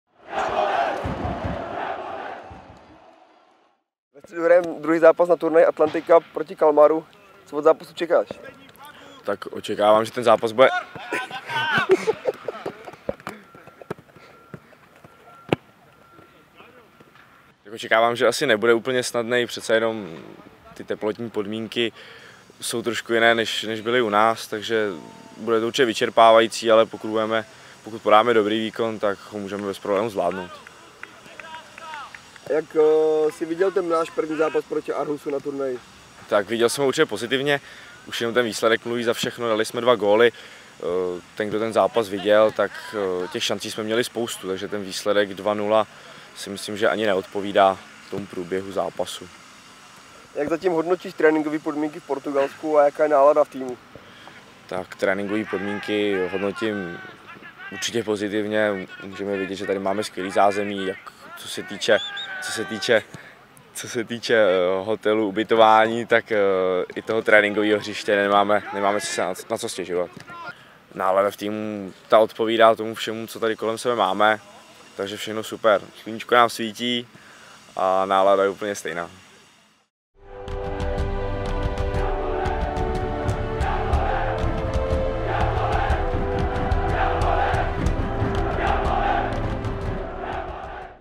Petr Rys vyhlíží souboj s FF Kalmar
Ve středu hrajeme druhé utkání na turnaji Atlantic Cup v Portugalsku a soupeřem nám bude švédský FF Kalmar. Jak se na zápas těší reprezentační obránce Petr Rys. Nutno dodat, že dělat rozhovory pro TV Střelnice před tréninkem není za přítomnosti dalších spoluhráčů vůbec jednoduché :-)